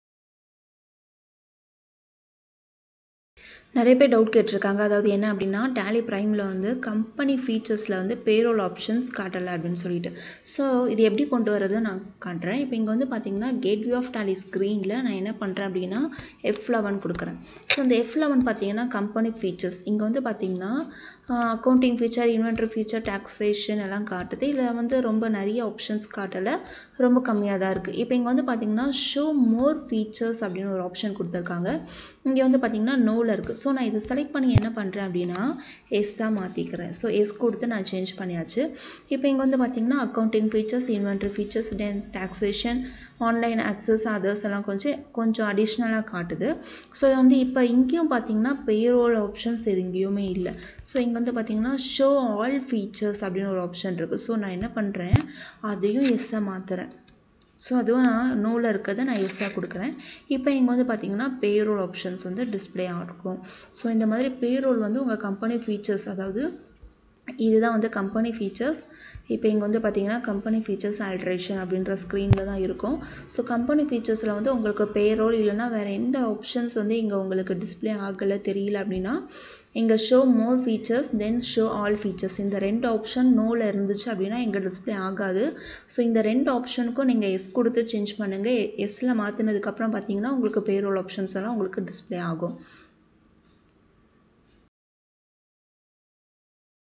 FAQ - How to enable Payroll in Tally Prime || Tally Prime in Tamil
Tally Prime Tutorial ( தமிழ் )
How to enable Payroll in Tally Prime
Santal Informatics Pvt Ltd
Tally Prime is the latest update of Tally ERP9. Learn the Complete Tally Prime Course here